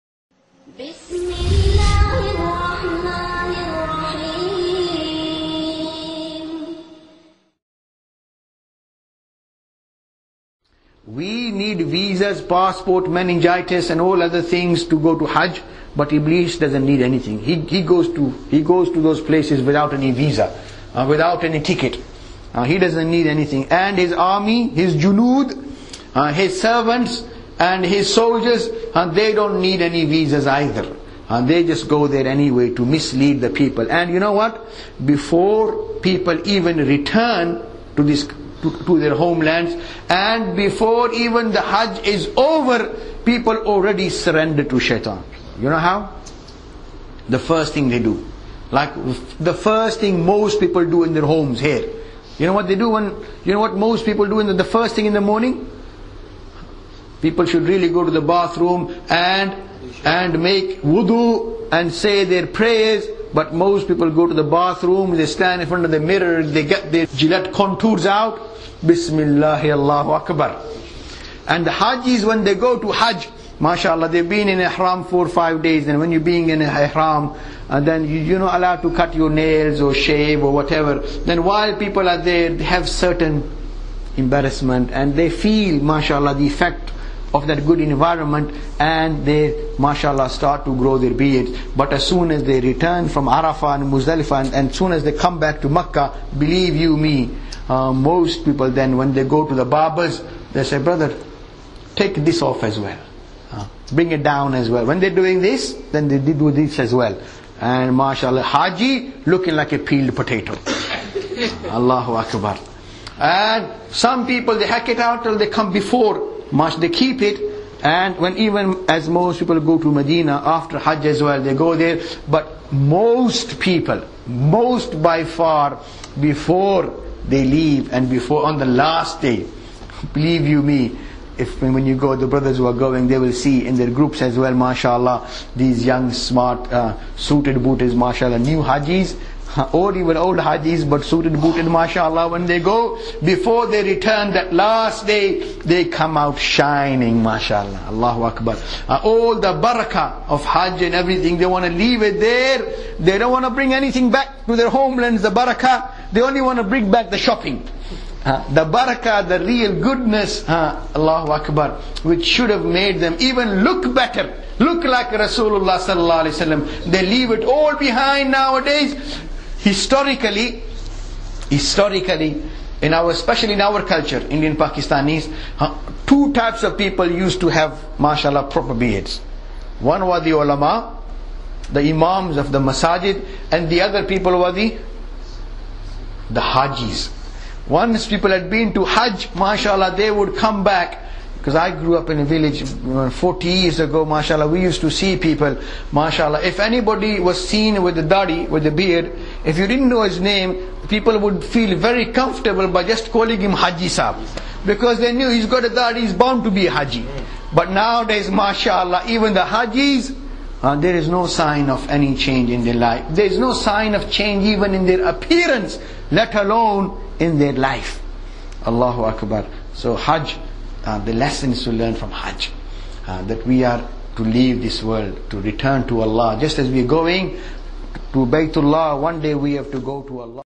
The 'Peeled Potato' syndrome HAJJ EDITION
From the creators of "The 'Peeled Potato' syndrome" we now bring you: The 'Peeled Potato' syndrome HAJJ EDITION

This is an excerpt from a soon to be released lecture called "Journey of a lifetime" by Sheikh Mumtaz ul haq.